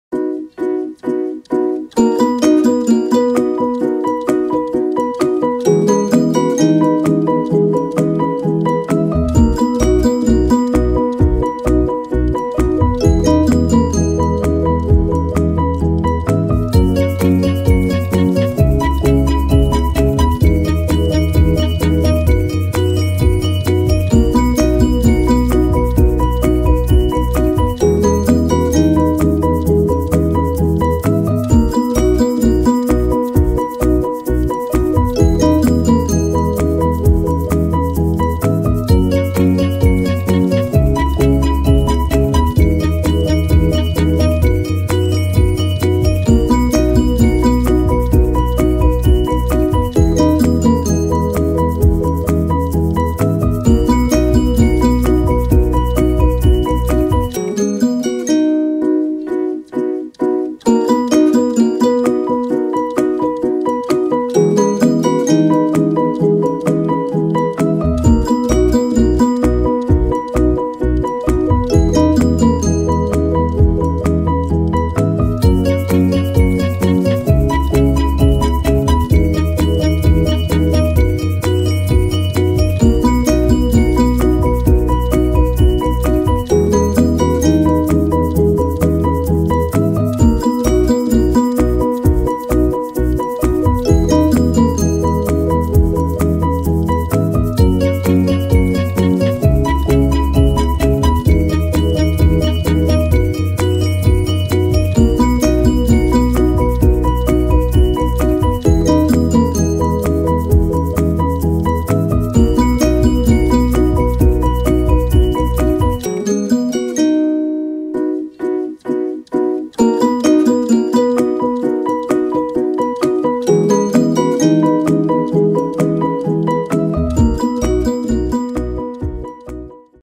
Quick and easy paper pinwheel quilt block video tutorial
This is another quilt block made with half square triangles and squares. This is an easy beginner quilt block.